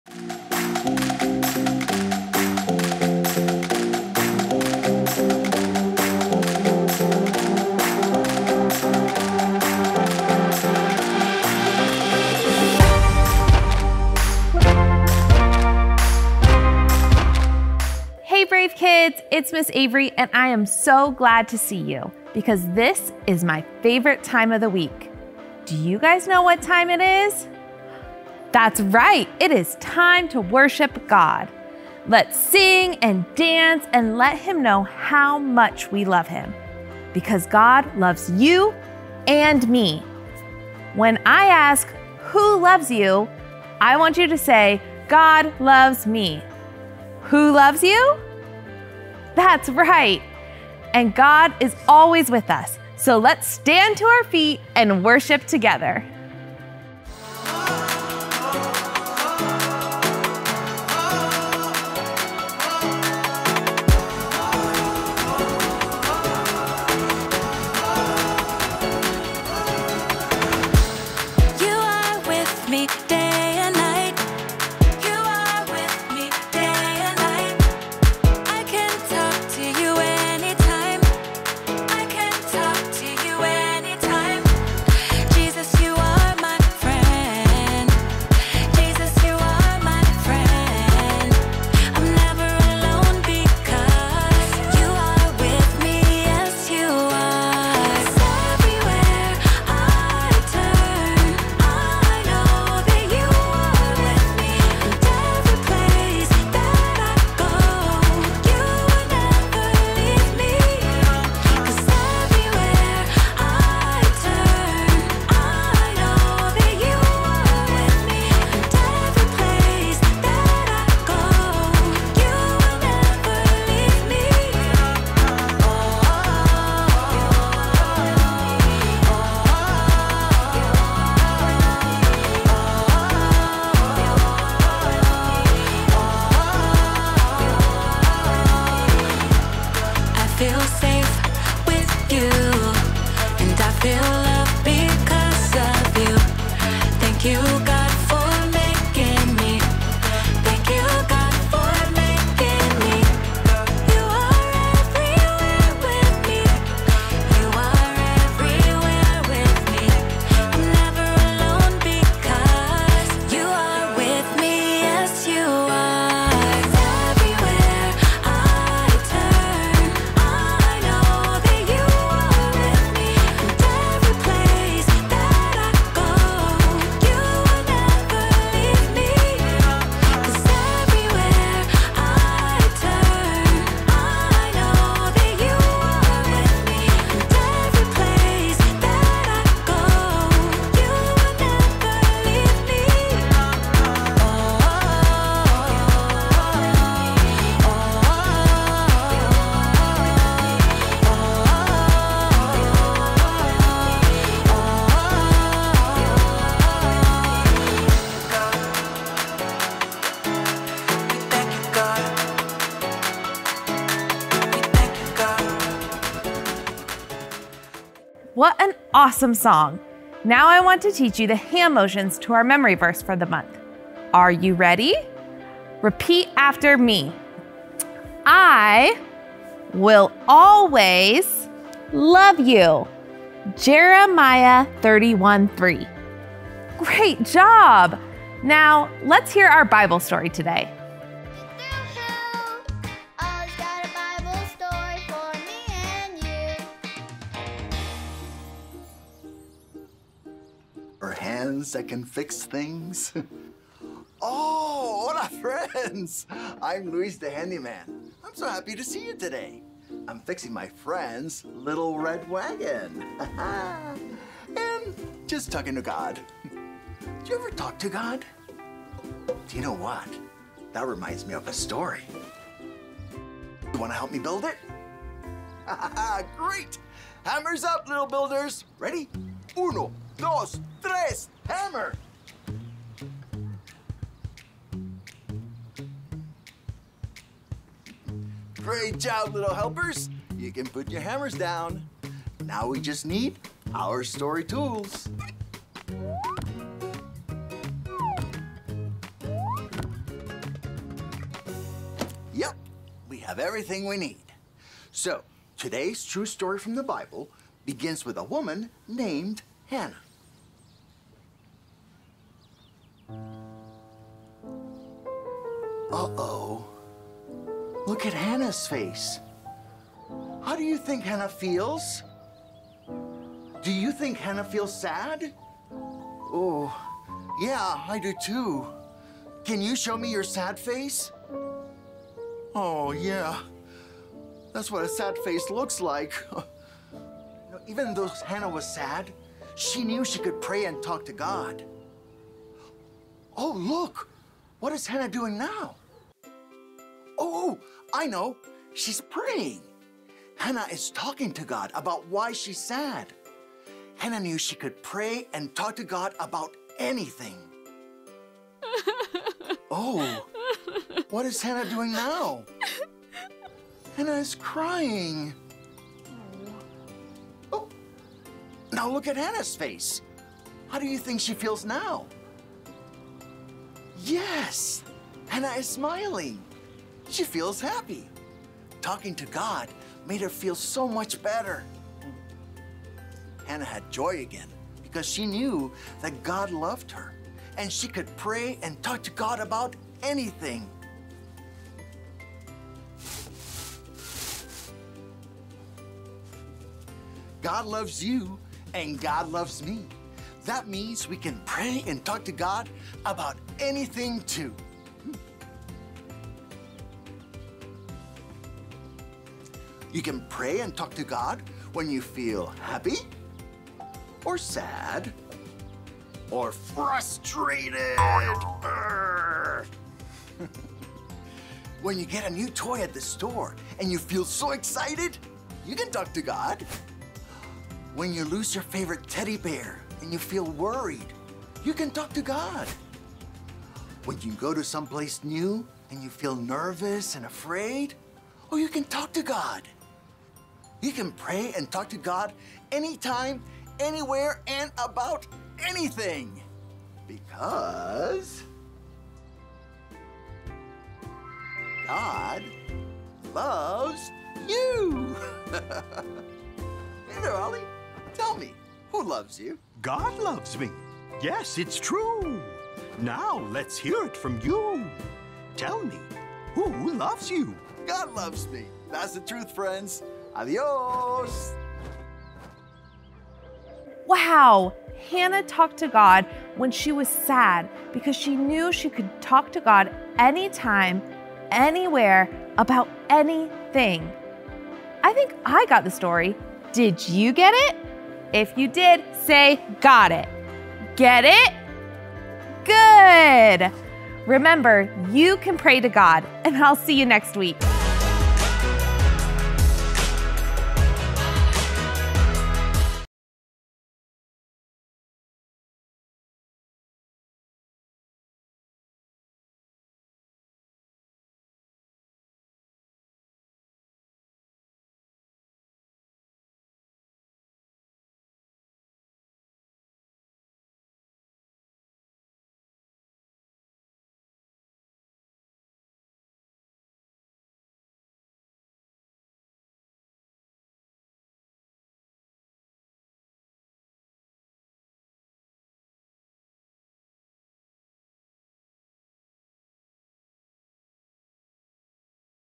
Hannah - Preschool May Week 2 | Brave Kids
Hannah talks to God when she is sad because she knows she can talk to God anytime, anywhere, and about anything. And after Hannah cries out to God and talks to Him about why she is so sad, she isn’t sad anymore. We want preschoolers to know that they can talk to God too! Anytime, anywhere, and about anything just like Hannah did, because God loves me!

1 Samuel 1-2

Thanks for joining us!